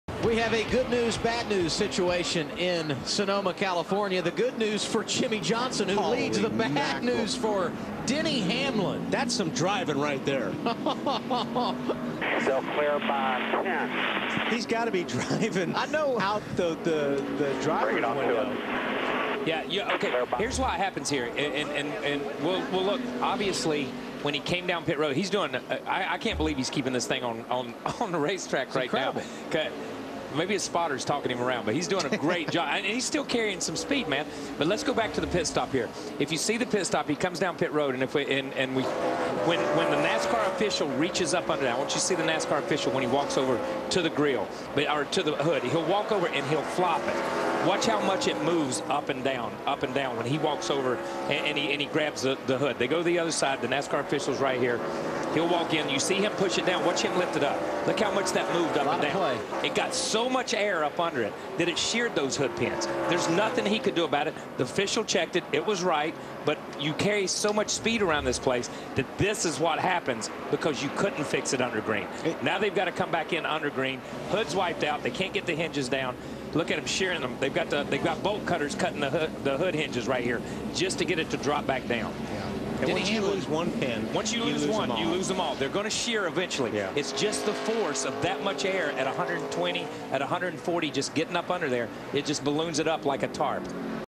Denny Hamlin Still Having a Bad Day Toyota Save Mart 350 Infineon Raceway Sprint Cup 2010.mpg
Denny Hamlin Still Having a Bad Day Toyota Save Mart 350 Infineon Raceway Sprint Cup 2010